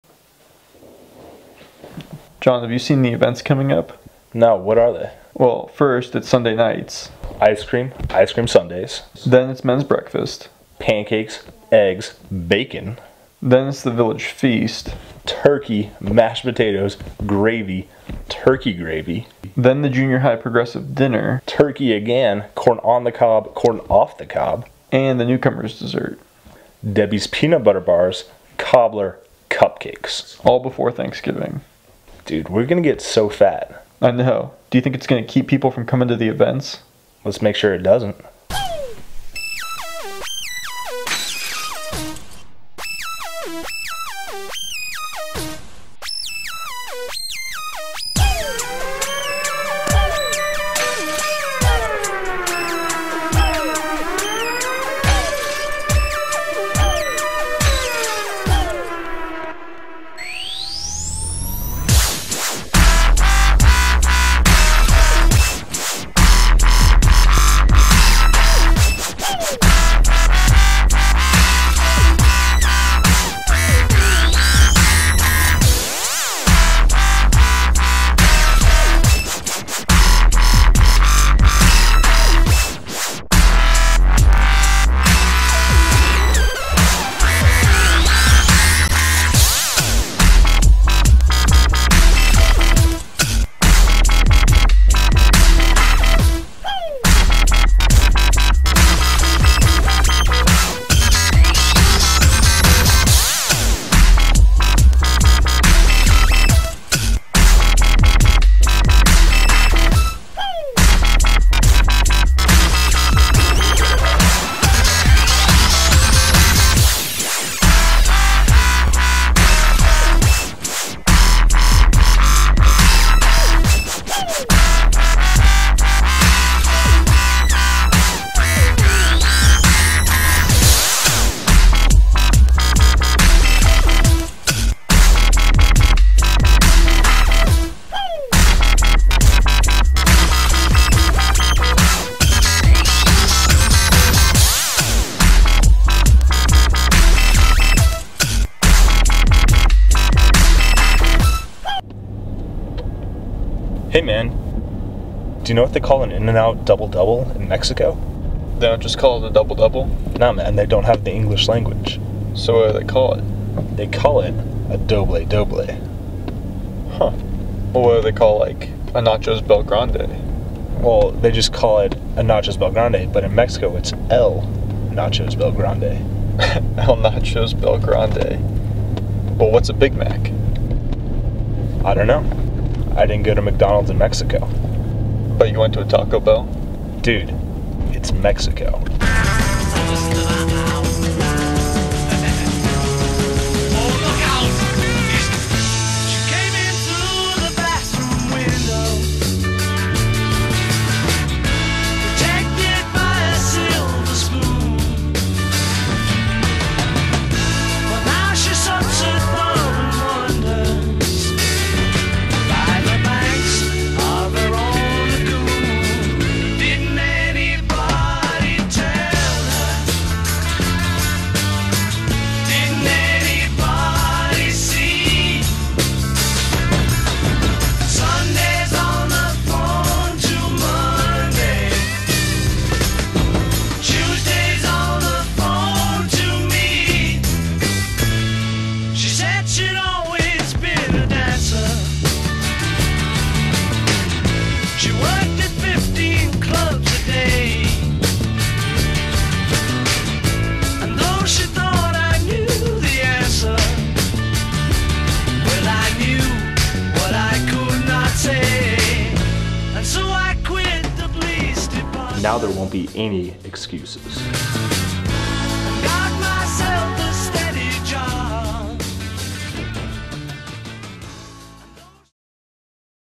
Scales Make You Fat
Rolling Hills has a lot of food oriented events coming up. We don't want anyone to miss them for fear of gaining too much weight, so we stole everyone's scales. Scales make you fat, after all.